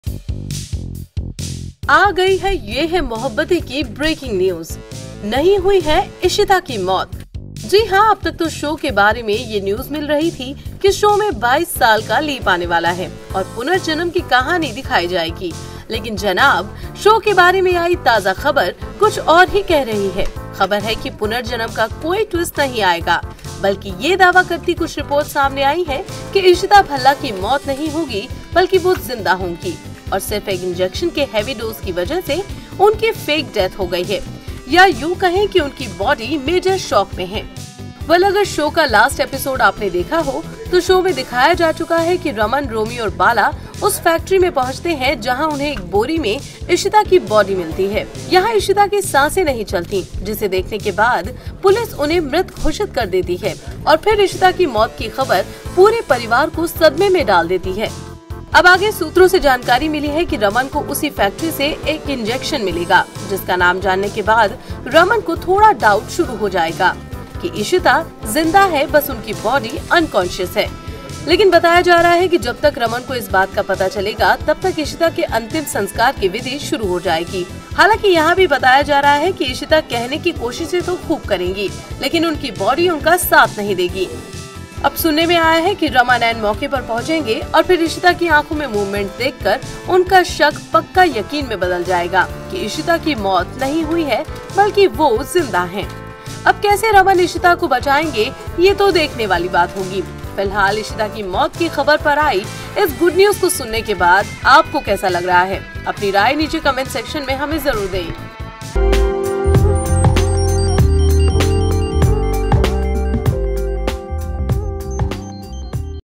BREAKING! Ishita is not Dead, but ALIVE in Yeh Hai Mohabbatein
The upcoming episode of Yeh Hai Mohabbatein is gearing up for some major drama..According to some latest news Ishita isn’t Dead, but alive while in a major body shock state.. Check Out the video